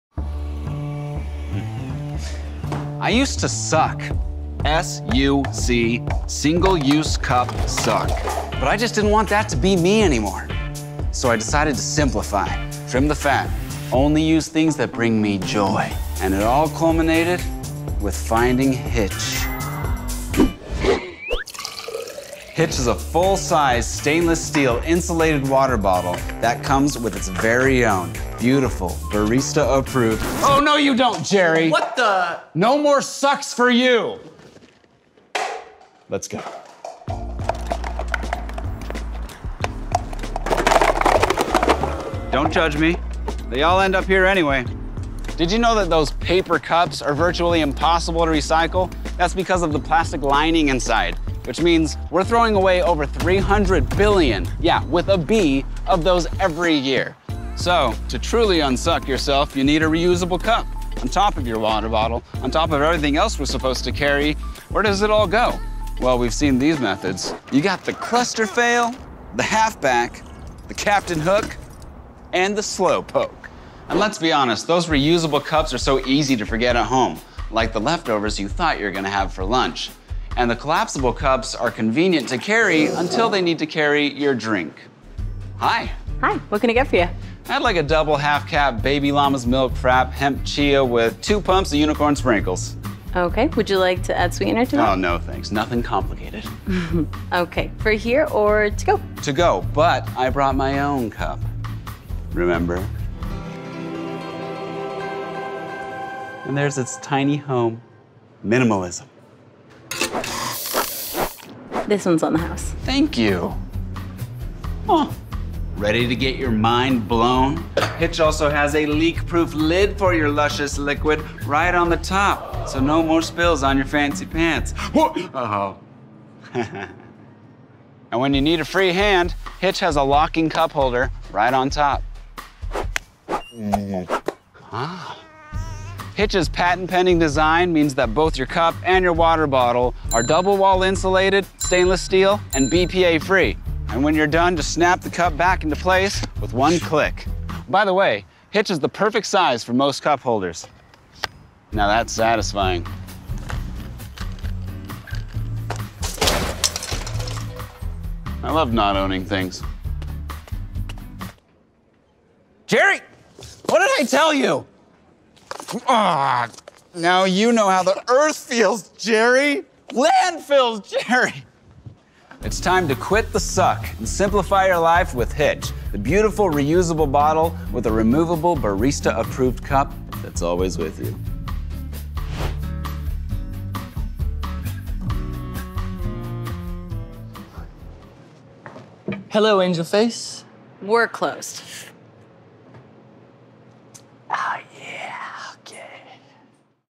Hitch - Your Bottle and Cup Belong Together
The water bottle with a removable barista-approved cup hidden inside. Go zero waste & carry better.

Hitch is the first water bottle with a removable barista-approved cup hidden inside. The bottle and cup each full sized (18oz bottle / 12oz cup), double walled, vacuum insulated, stainless steel, leak-proof, and BPA free. With a bottle you'll love, and a cup that's always with you, go zero waste and carry better.
Hitch's patent-pending design makes it easy to carry your bottle and cup together. Simply turn the crossbar on the bottom, and the cup releases from the bottle.
When we set out to create Hitch, we talked to lots of people who carry reusable water bottles, and most wouldn't carry a reusable cup because it was either too much too carry everything, or too easy to forget at home.

Simply making a tiny water bottle and tiny cup wouldn't cut it--people wanted a full size water bottle for all day hydration, a reusable cup large enough for a standard coffee drink with a lid that won't leak, and a way to carry both that was space efficient and easy to hold.  In fact, we wanted it to be so easy, that people could go zero waste.
Single-use paper cups and plastic bottles create a lot of waste. With Hitch, we've made the reusable bottle and cup so easy to carry, that you can go zero waste and lighten your load.